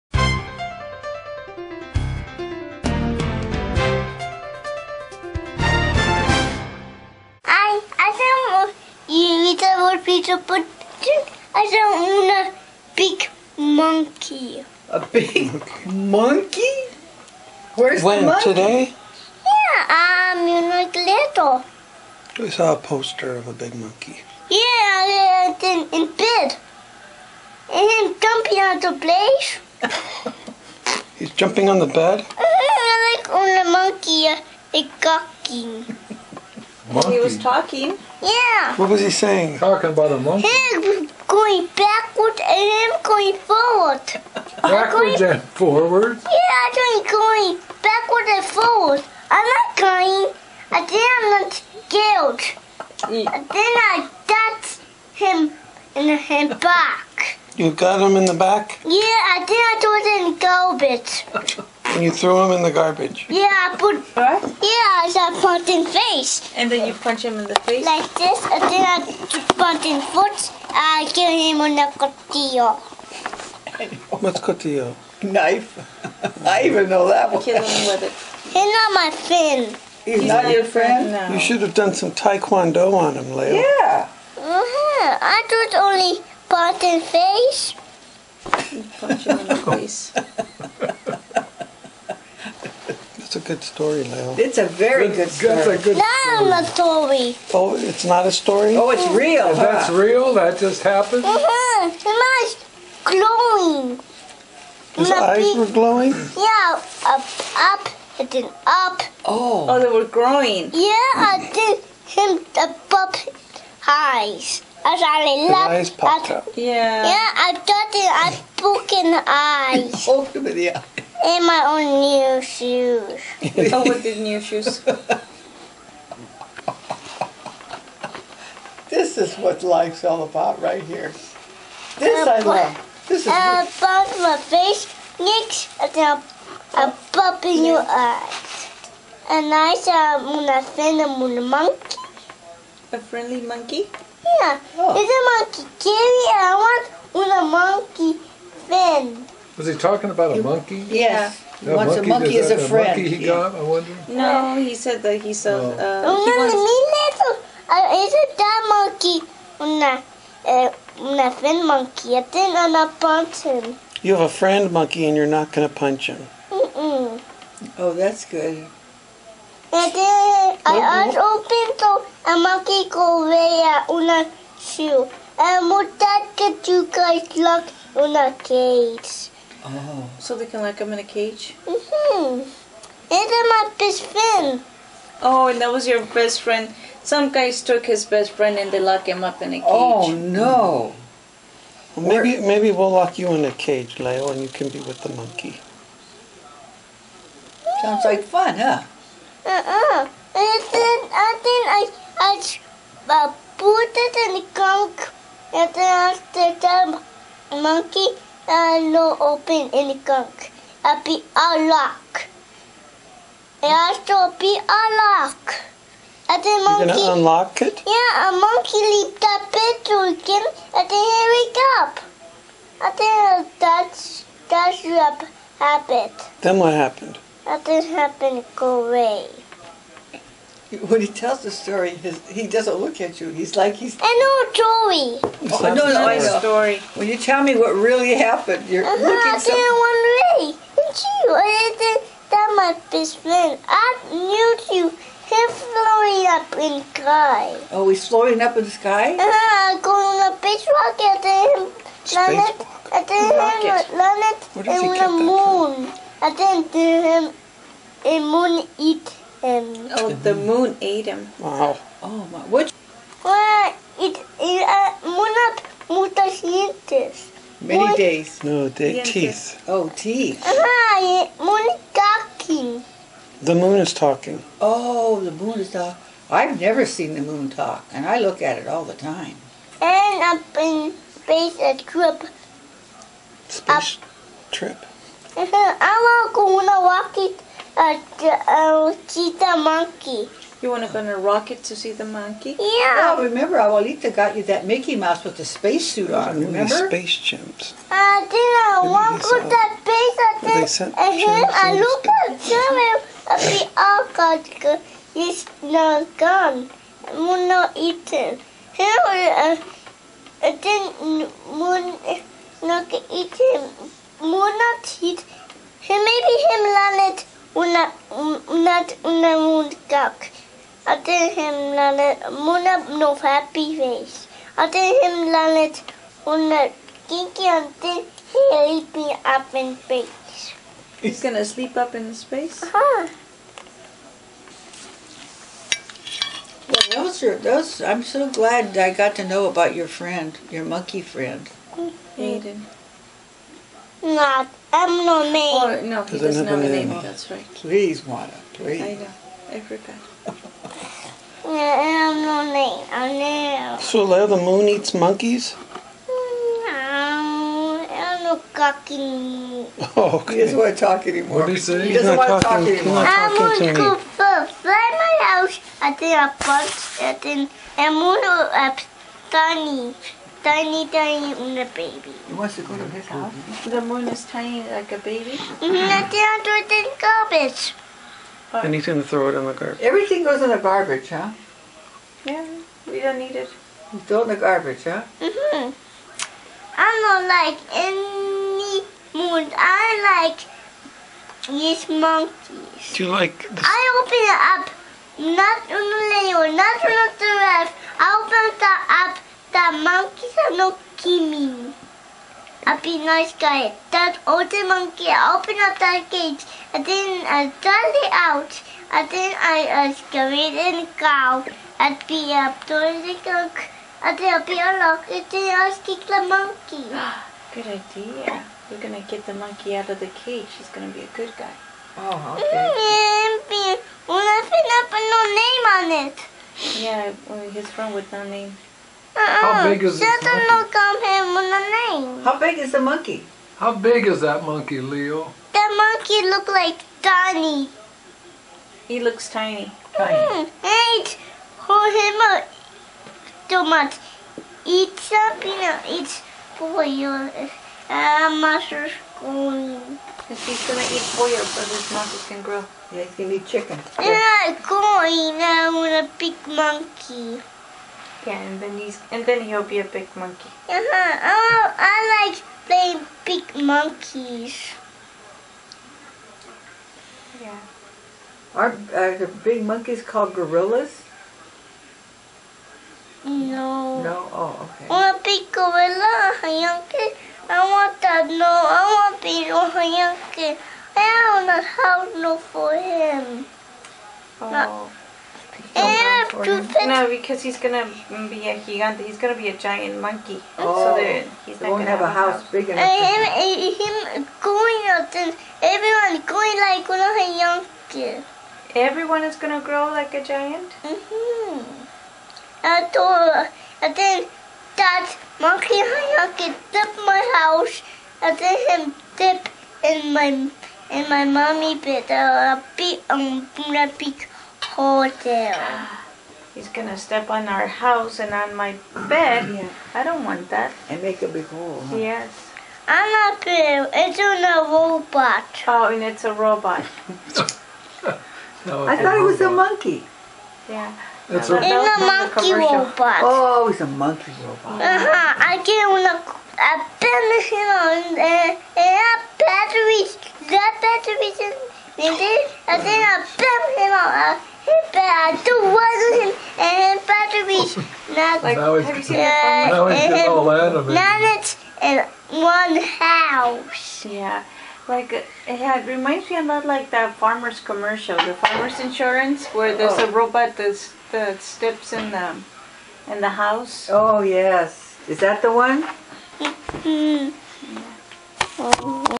Robot Monkeys on the Moon
Leo tells a funny story